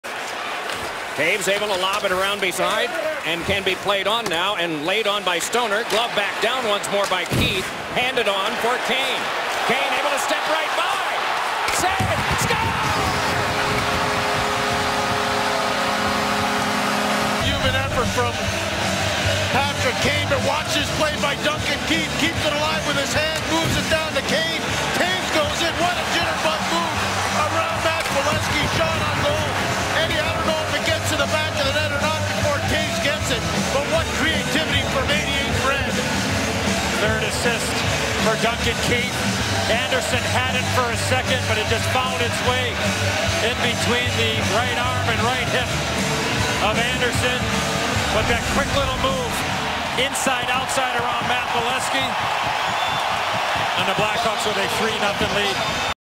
Kane shows off incredible skills to beat Andersen
Chicago Blackhawks forward Patrick Kane shows off his amazing stick skills to get past the Ducks defense and score on goalie Frederik Andersen.